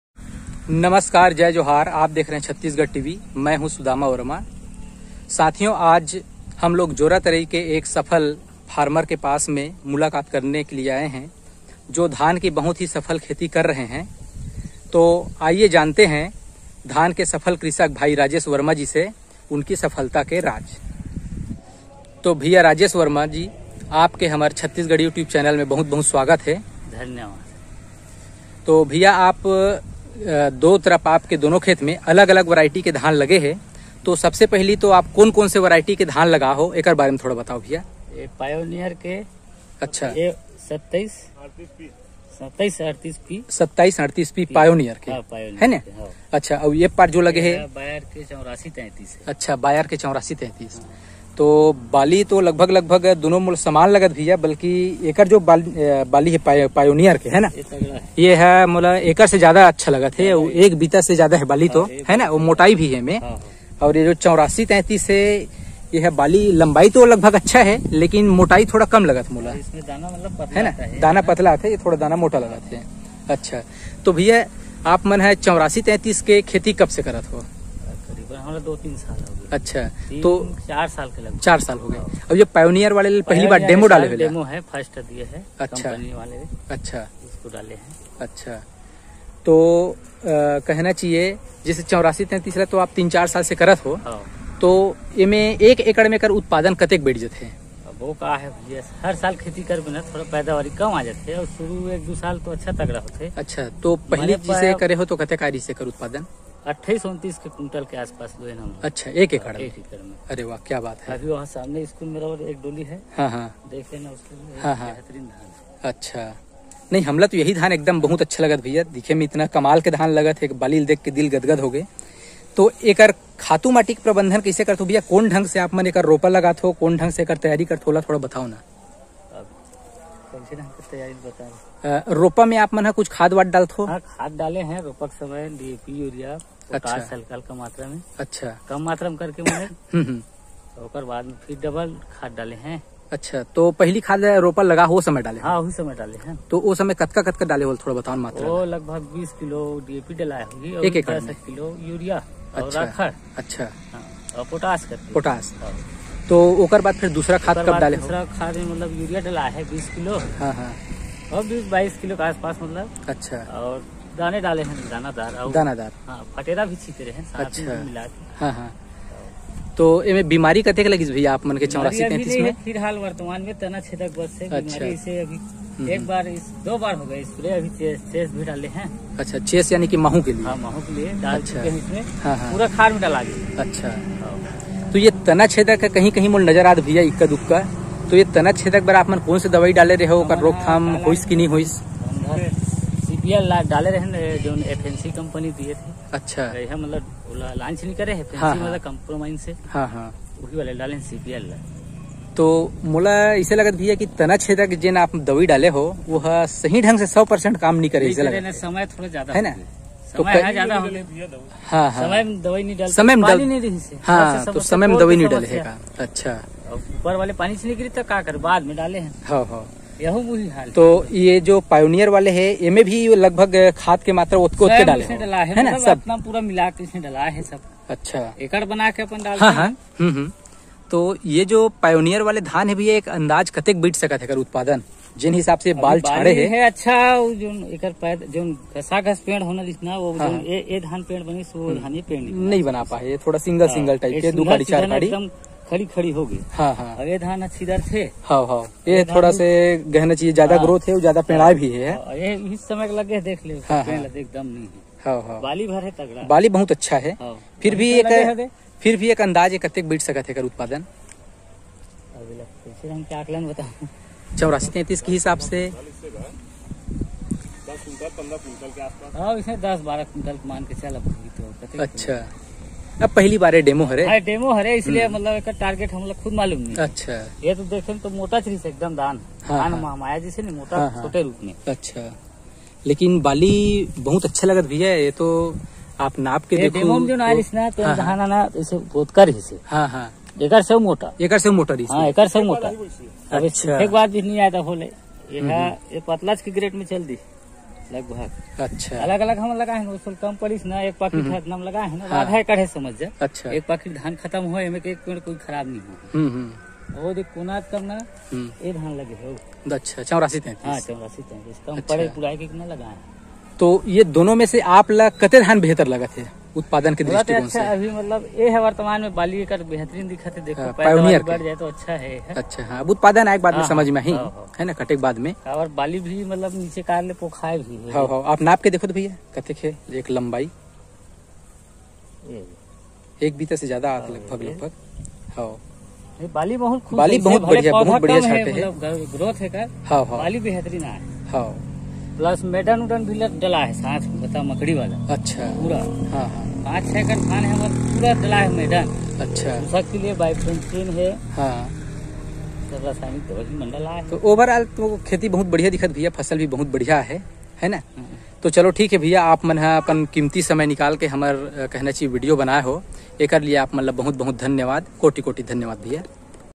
धान की उन्नत खेती धान के सफल किसान राजेश वर्मा छत्तीसगढ़ टीवी
राजेश वर्मा के द्वारा धान की सफल खेती छत्तीसगढ़ टीवी